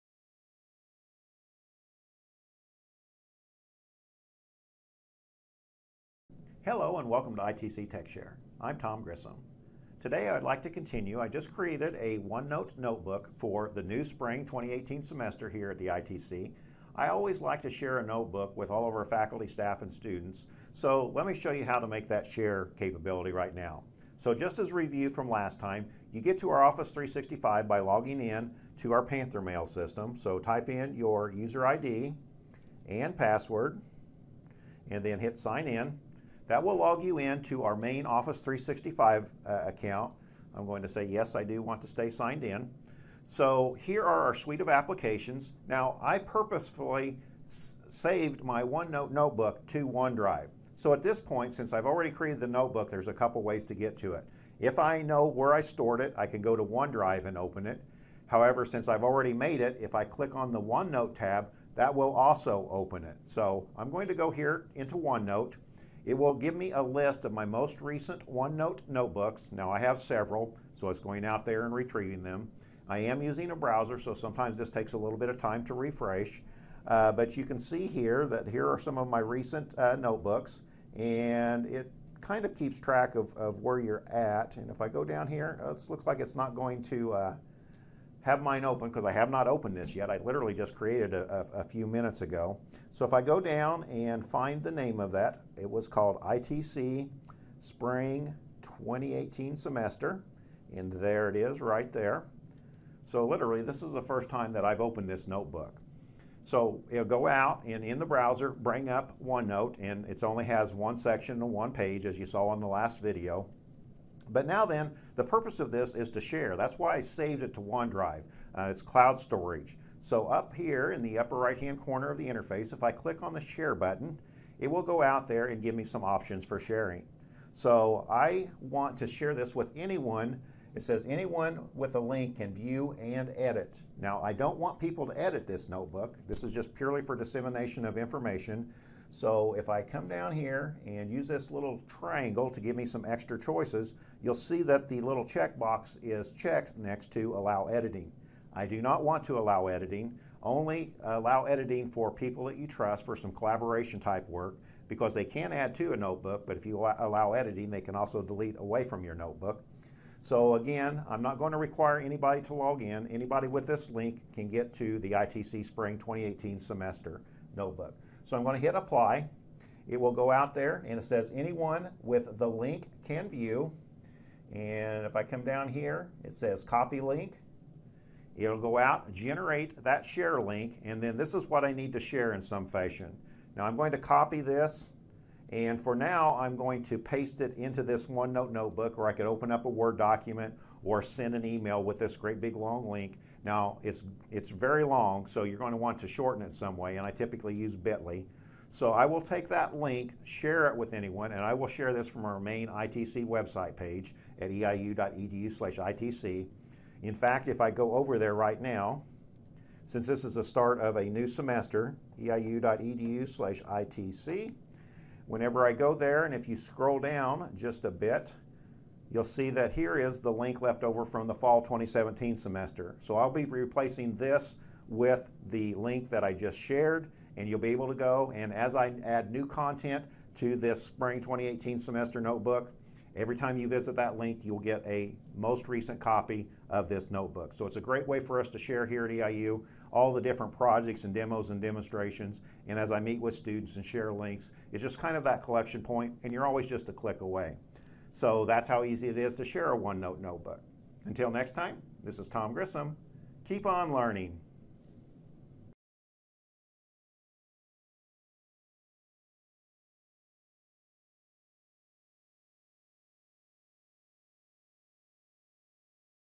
itcts 083 - Share a OneNote Notebook as View-Only
01192018 ITC Techshare - Share a OneNote Notebook as View-Only
Think of OneNote as a digital three ring binder that allows you to take notes and organize your life. Works on PC, Mac, Linux as well as apps for iOS and Android smart phones.
-tg